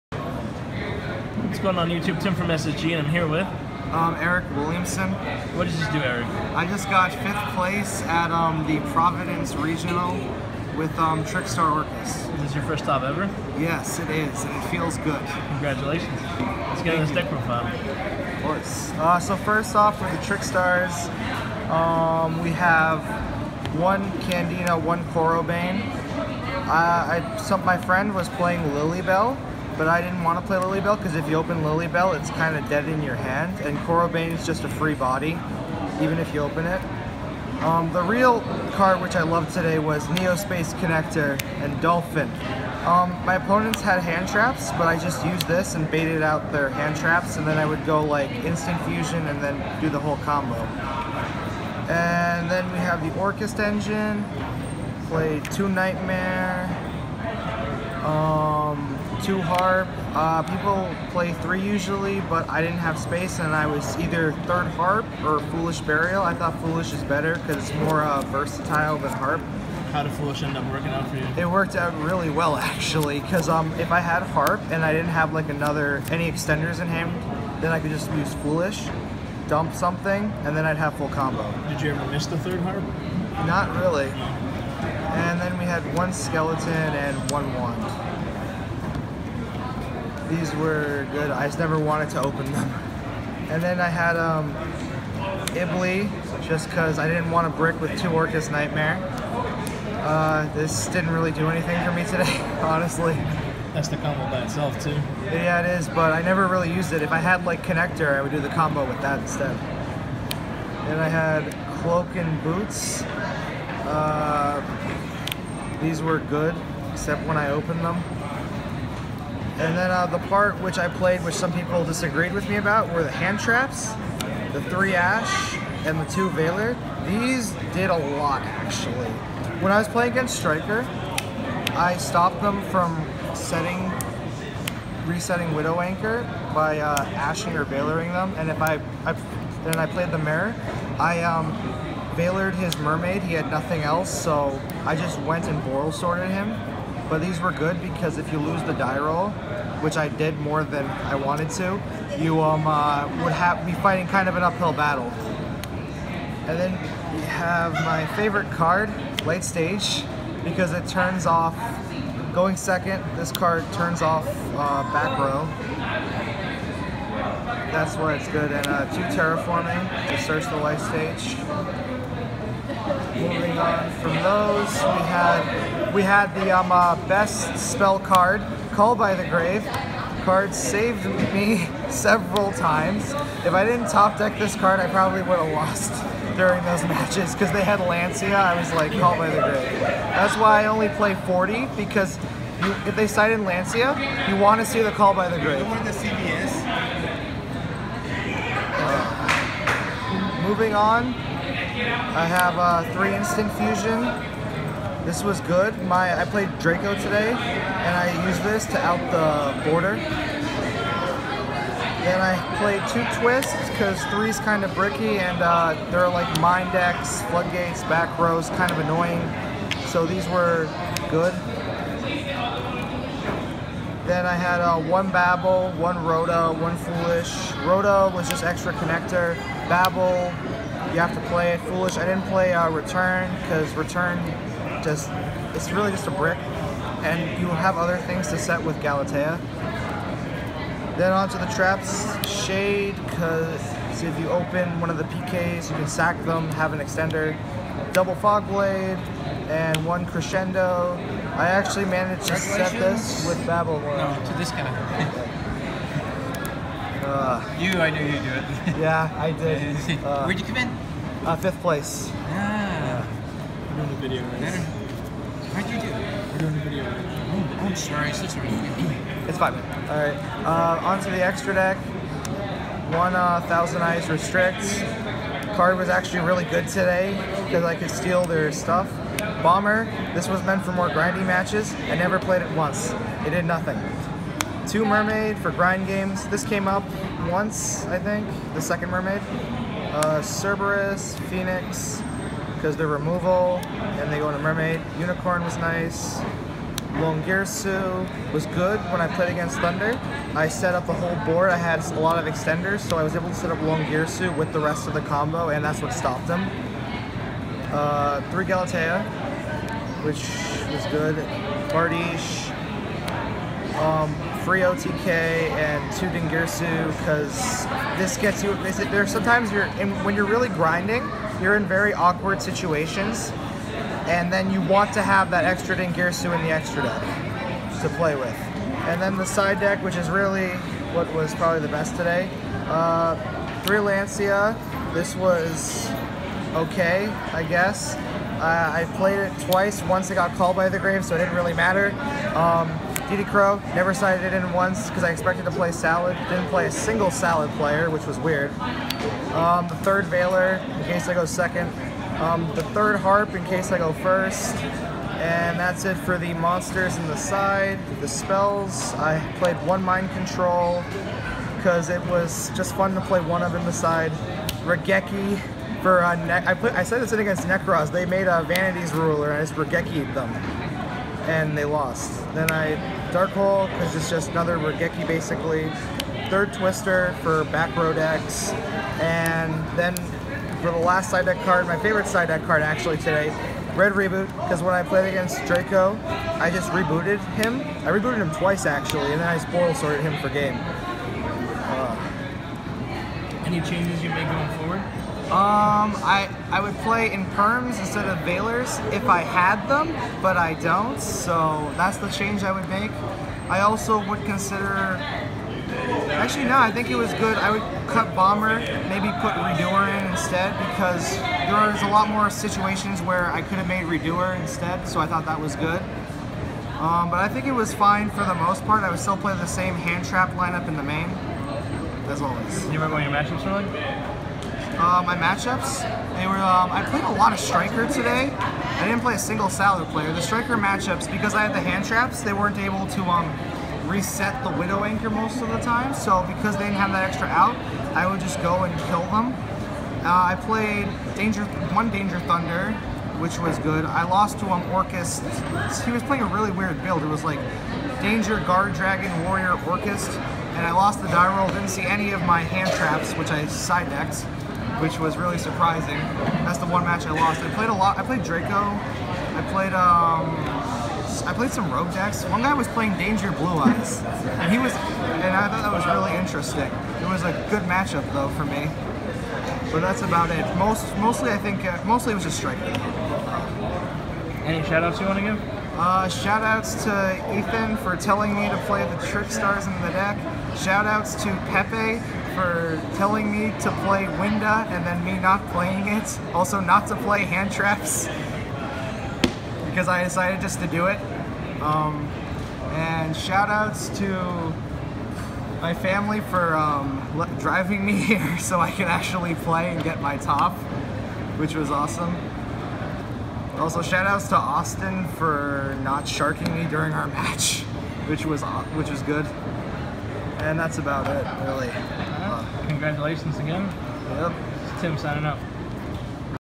Yu-Gi-Oh! 5th place Orcust Regional Deck Profile Providence RI 2019
Hey everyone hope you enjoyed this deck profile. Shout out and congratulations to Eric Williamson on the 1st top 8 finish and deck profile .